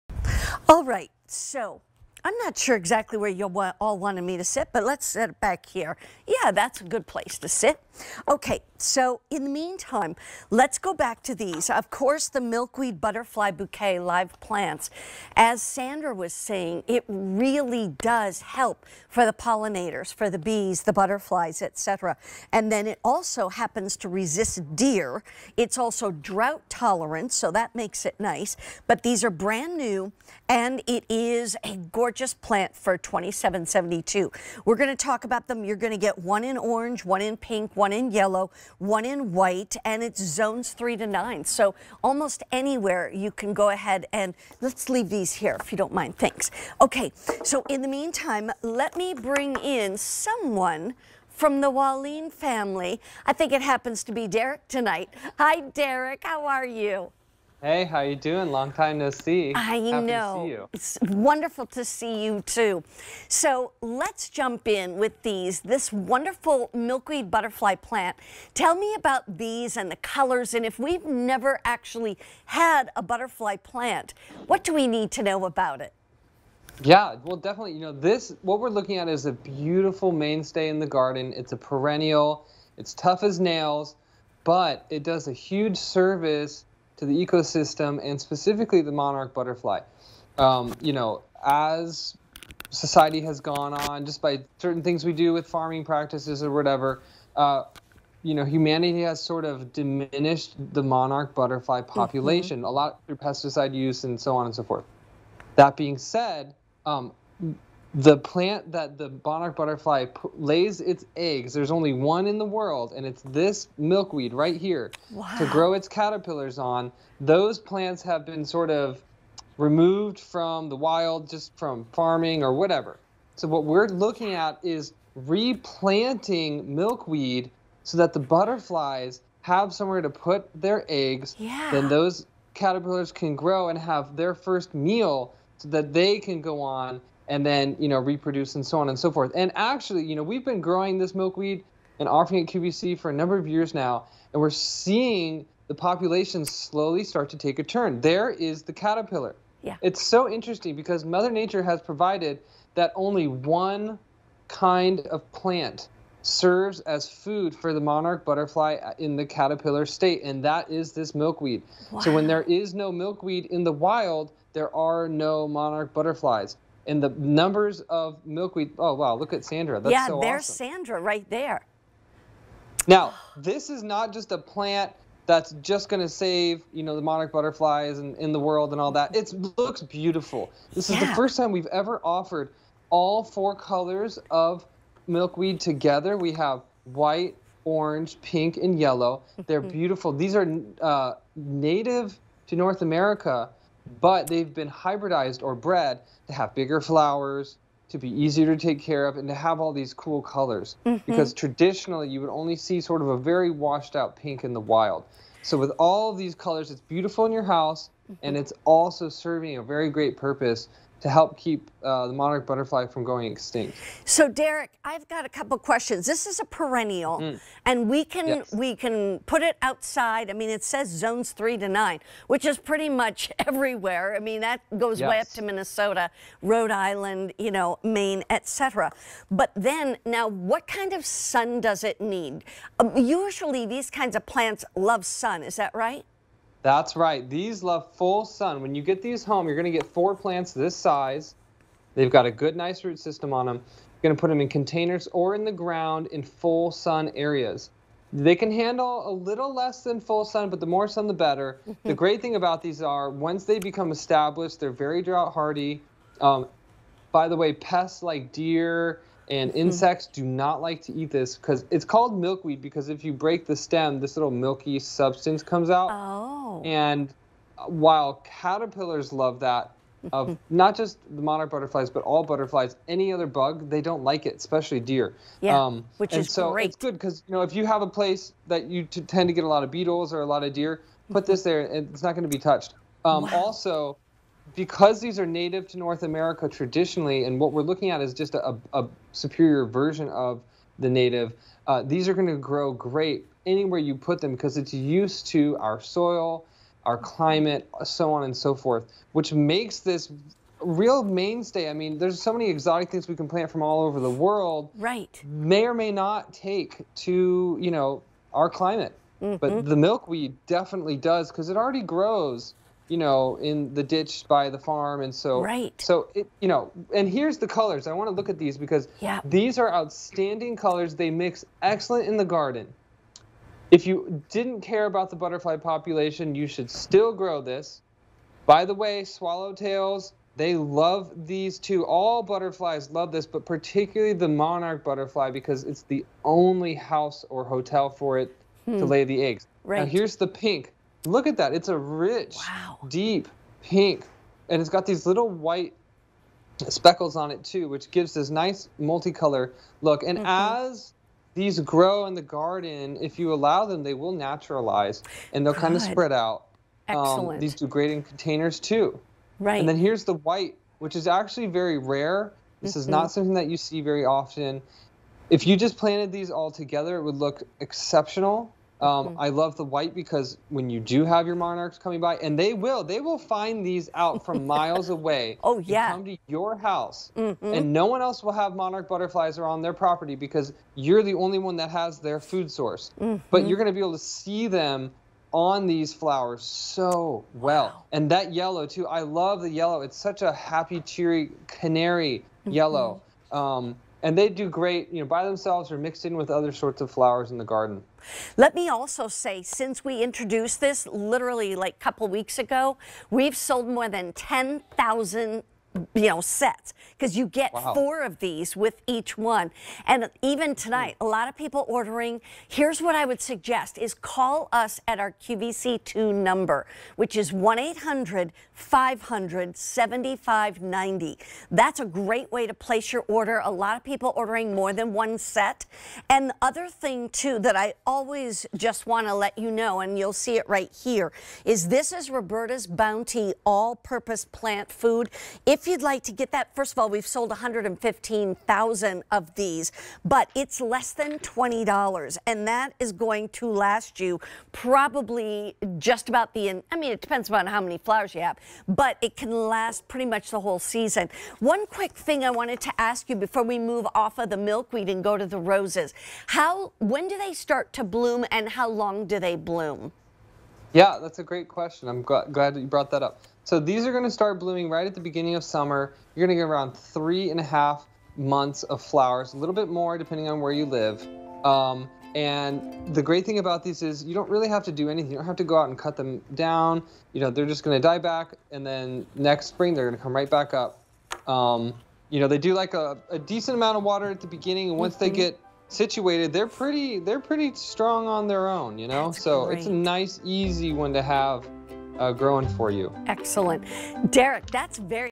Roberta's 4-pc Milkweed Butterfly Bouquet Live Plants on QVC
Roberta's 4-pc Milkweed Butterfly Bouquet Live Plants

These easy-to-grow perennial plants act as a host for Monarch butterflies and brightens your garden with a colorful display. From Roberta's.